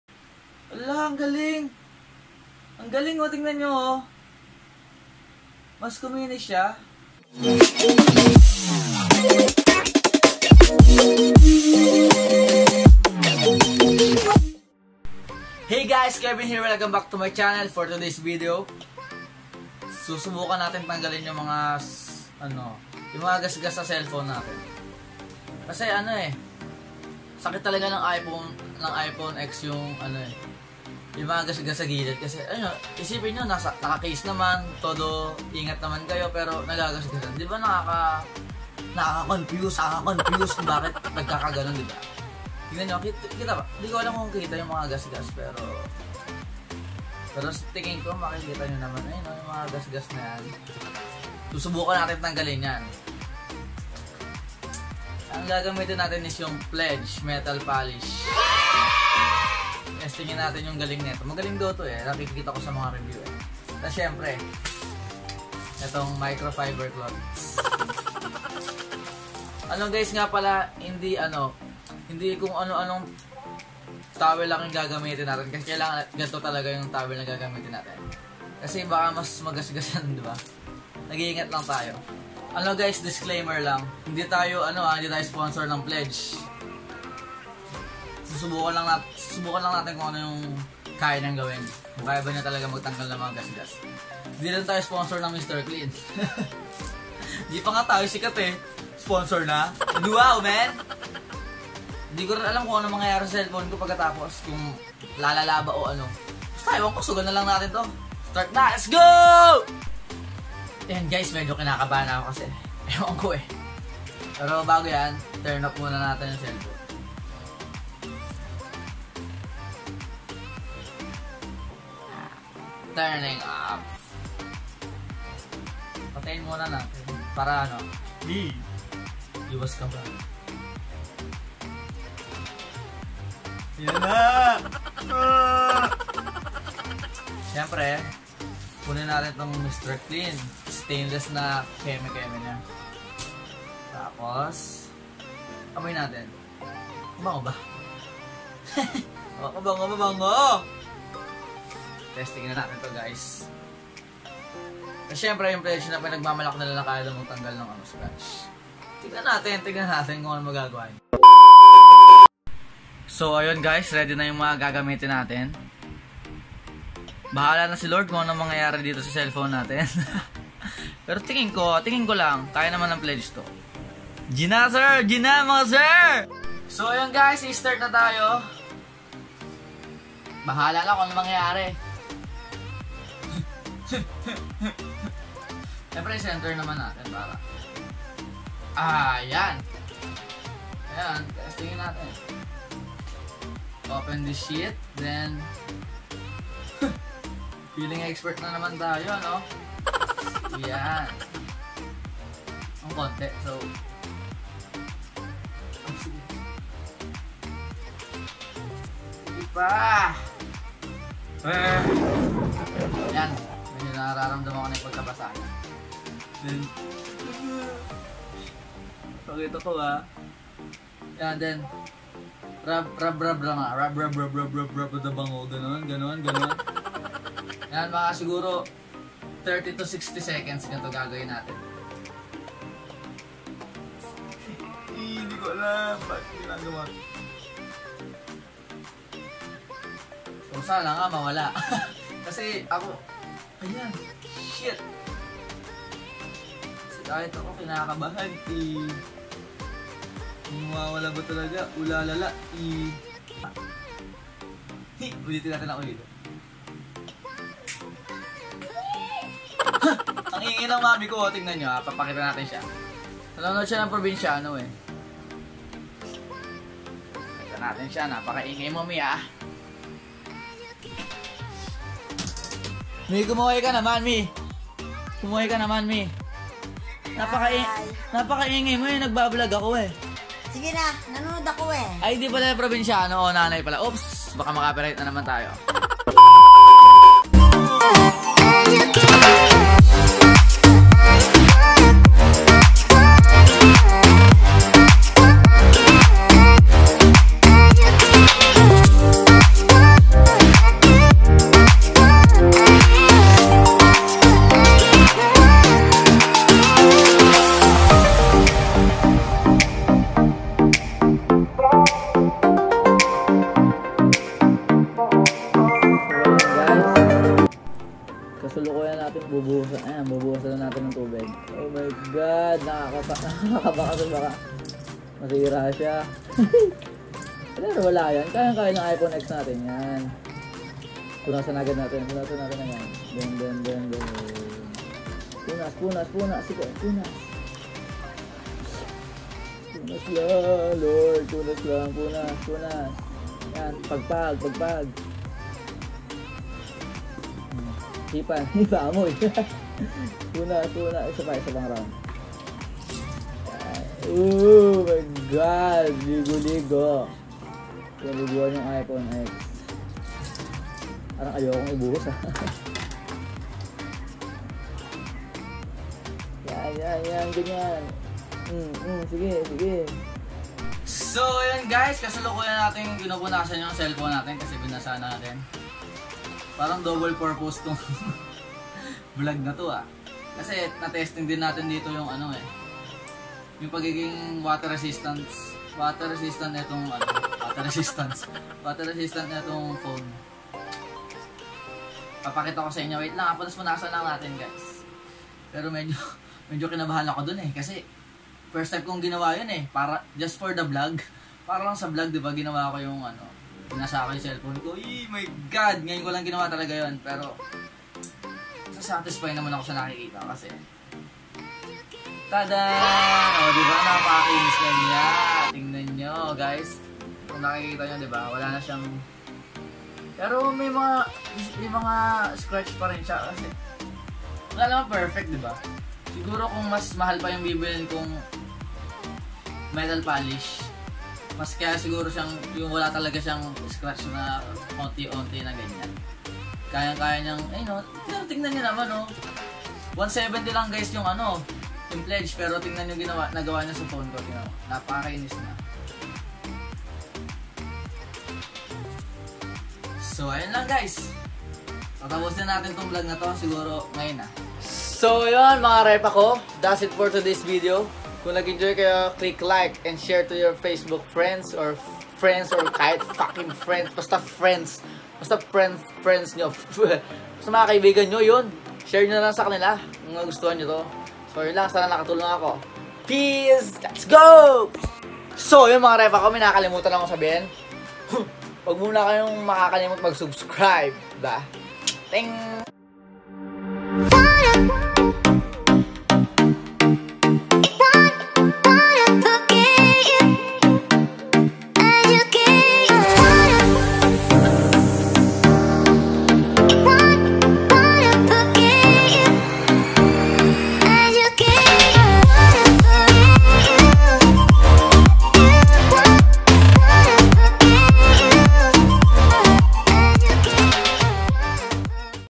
HOW TO REMOVE SCRATCHES FROM IPHONE X SILVER EASILY (Problem Solved)
Hello mga ka repa ko! Subukan niyo na to hinding hindi kayo mag si-sisi hehehe para siyang magic na mawawala nalang bigla yung mga scratches nya. Thank you guys for watching! I hope you enjoy! :)